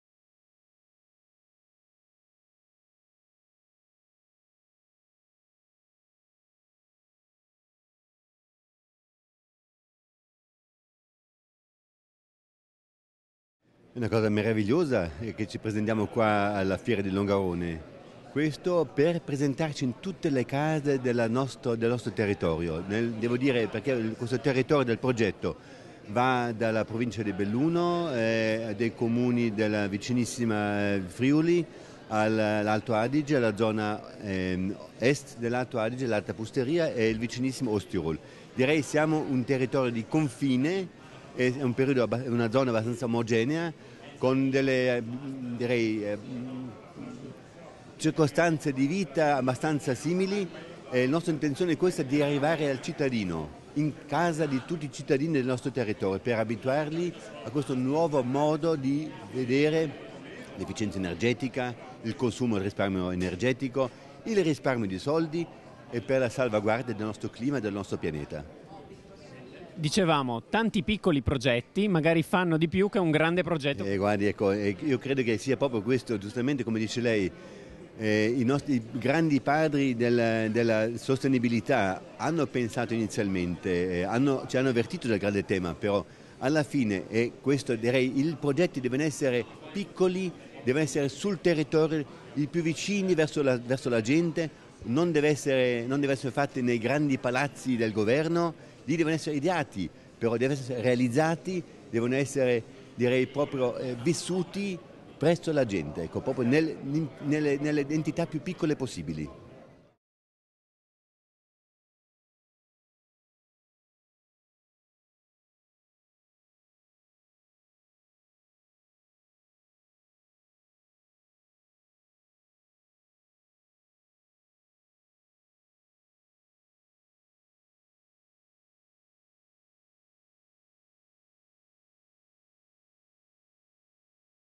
Neutralità climatica in fiera a Longarone: intervista ad Hans Schmieder
Piccoli progetti diffusi in efficienza energetica portano a grandi risultati, salvare il clima fa anche risparmiare soldi: intervista ad Hans Schmieder dell'Accademia dei Colloqui di Dobbiaco sul progetto Neutralità climatica - Premio per l'efficienza energetica in edilizia 2012 alla fiera Costruire di Longarone 2013.

Il Premio per l'efficienza energetica in edilizia 2012 è istituito dal Comune di Zoppola (PN) e i Comuni di Agenda 21 locale La Clanfe (composto dalle amministrazioni di Arzene, Casarsa della Delizia, San Giorgio della Richinvelda, San Martino al Tagliamento, San Vito al Tagliamento, Valvasone e Zoppola), di concerto con l'Accademia dei Colloqui di Dobbiaco (BZ), la Provincia di Belluno, il Consorzio BIM "Piave" e l'Associazione per la Pianificazione Territoriale Sillian (Austria).